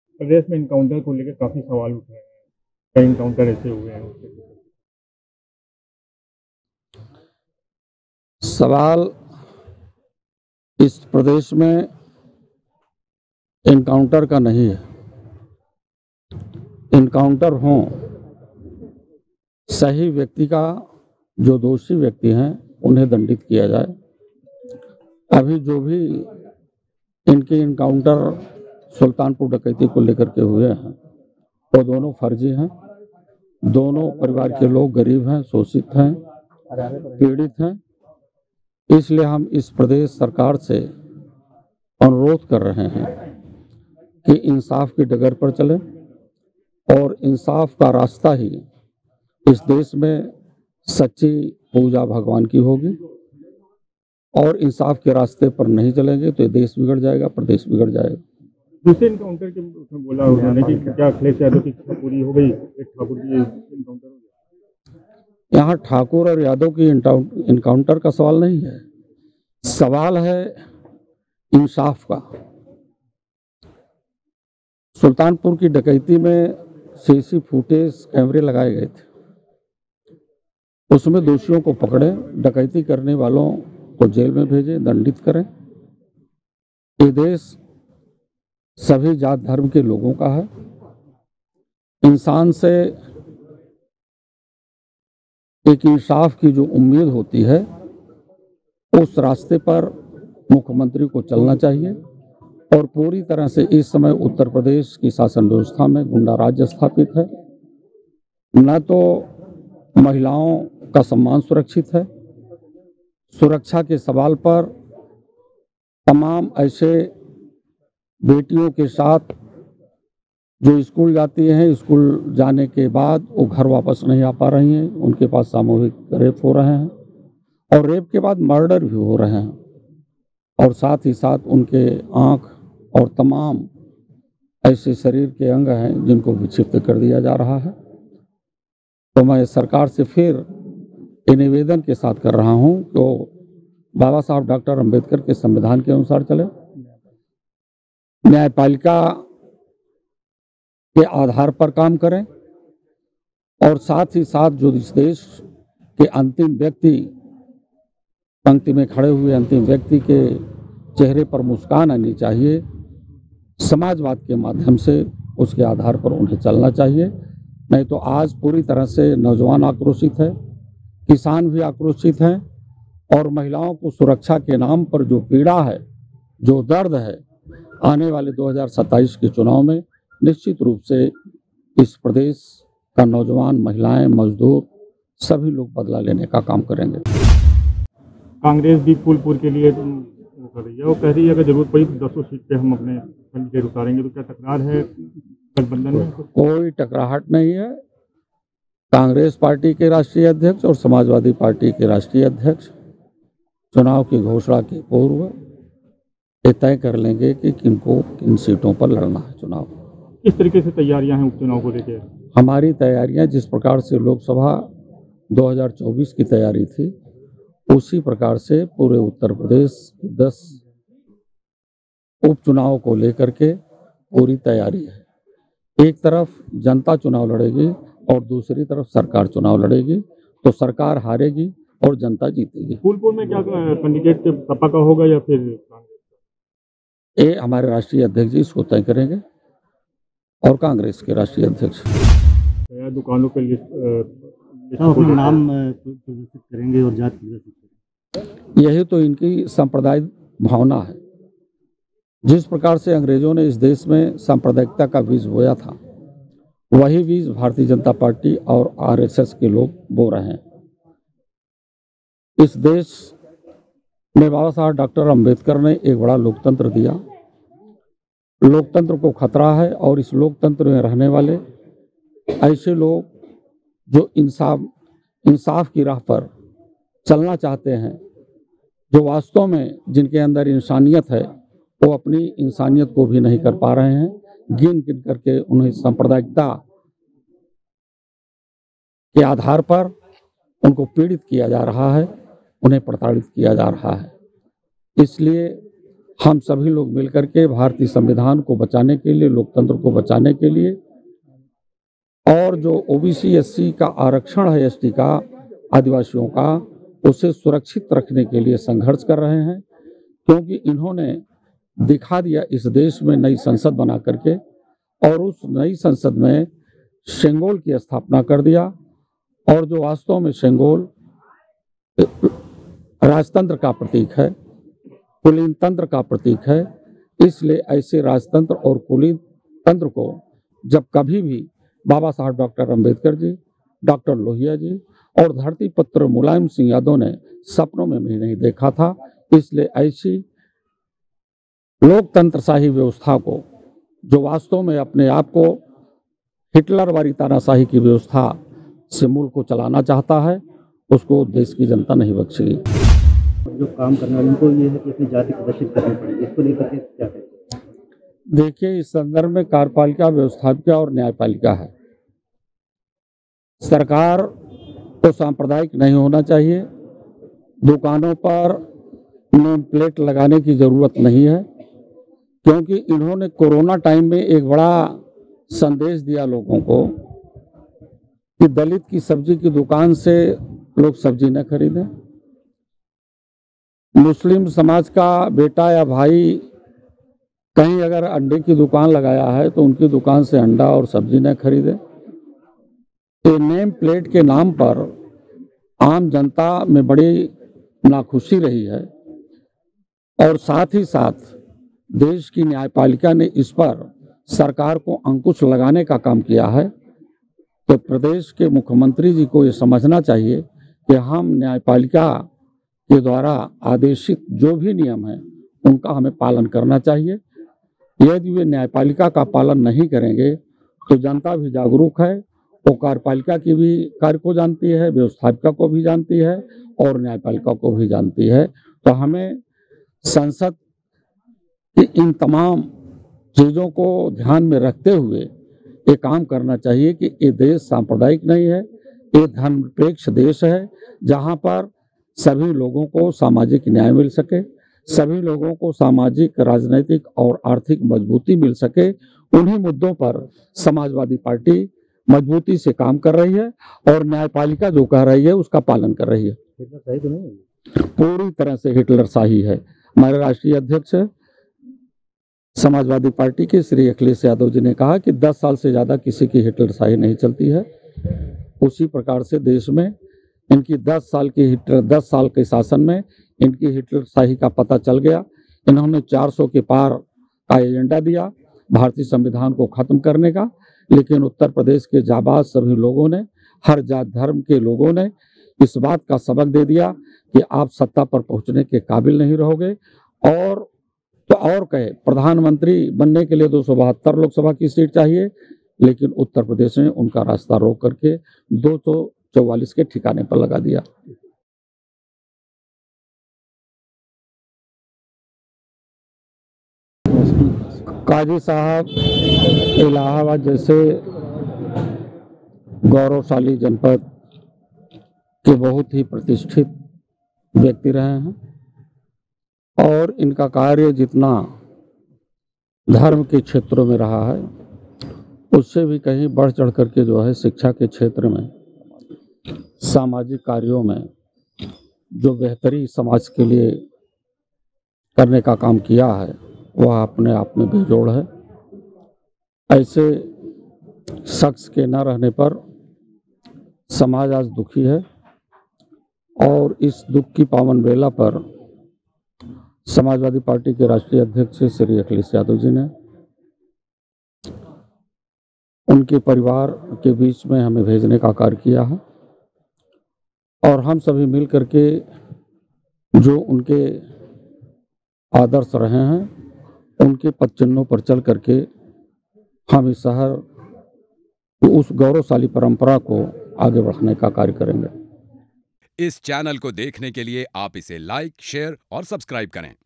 UP By-Polls 2024- फर्जी एनकाउंटर पर सपा के प्रदेश अध्यक्ष श्यामलाल पाल ने दिया बड़ा बयान। News Case
दुकान के आगे दुकानदार के नाम पता लिखे जाने वाले मामले पर समाजवादी पार्टी के प्रदेश अध्यक्ष श्यामलाल पाल ने कहा कि इसके संबंध में कार्यपालिका, व्यवस्थापिका और न्यायपालिका है सरकार को सांप्रदायिक नहीं होना चाहिए दुकानों पर नेम प्लेट लगाने की आवश्यकता नहीं है नेम प्लेट के नाम पर आम जनता में नाखुशी रही है देश के न्यायपालिका ने इस पर अंकुश लगाने का काम किया था इसलिए मुख्यमंत्री को समझना चाहिए कि न्यायपालिका के द्वारा आदेशित जो भी नियम है उसका पालन करना चाहिए